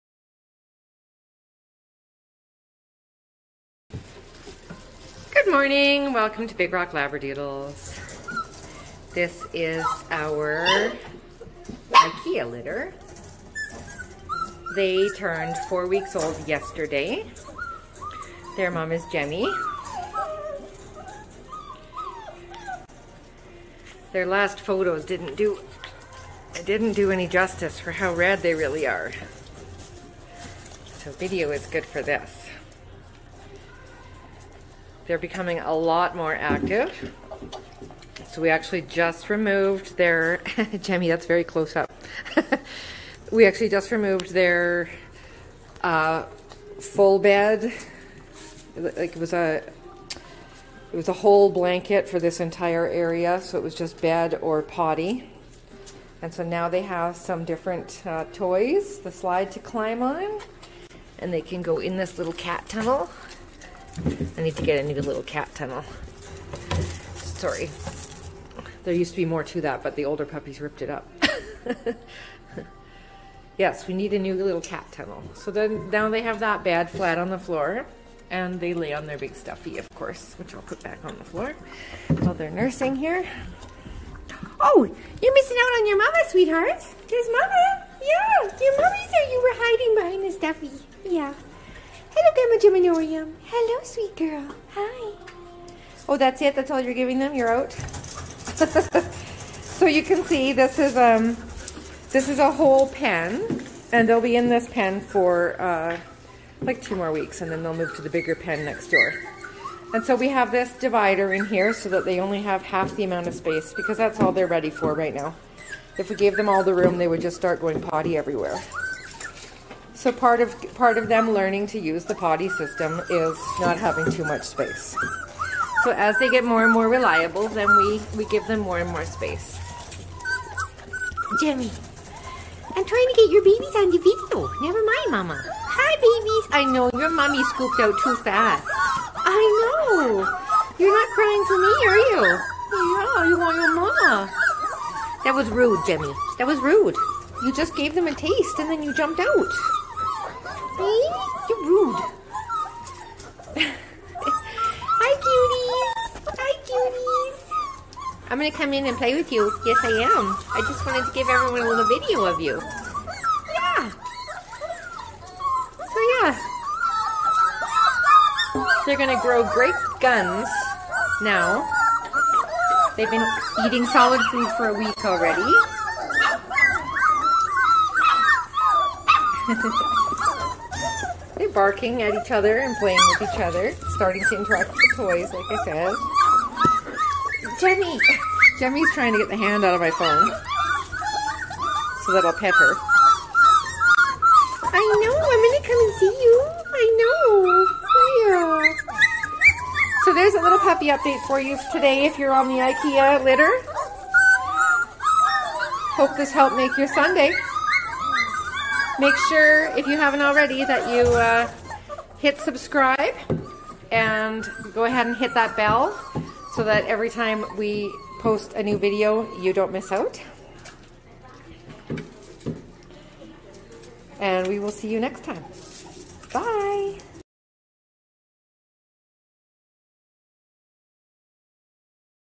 4 week IKEA update video
Happy Sunday, October 24th.  Our IKEA Litter is 4 weeks old..come along as I check in with them!